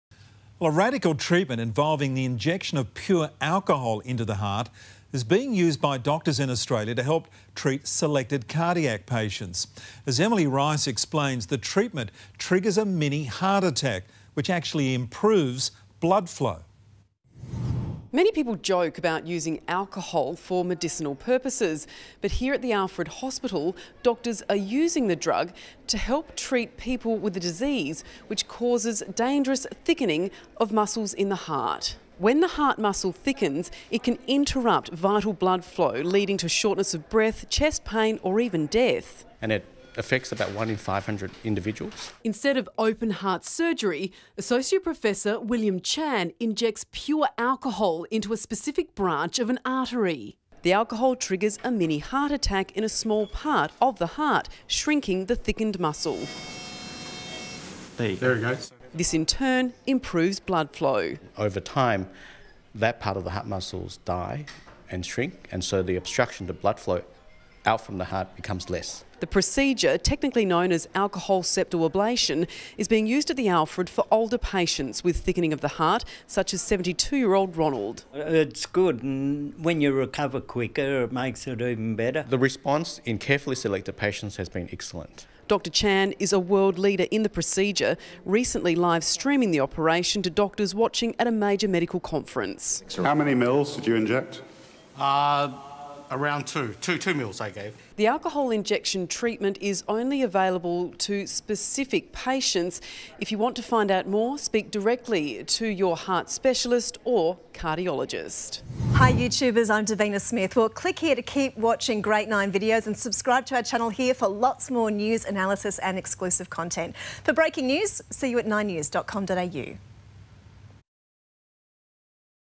How injecting alcohol into hearts cam actually save lives | Nine News Australia
It's well known that drinking too much alcohol can cause cardiac problems but injecting it straight into the heart, can actually save lives.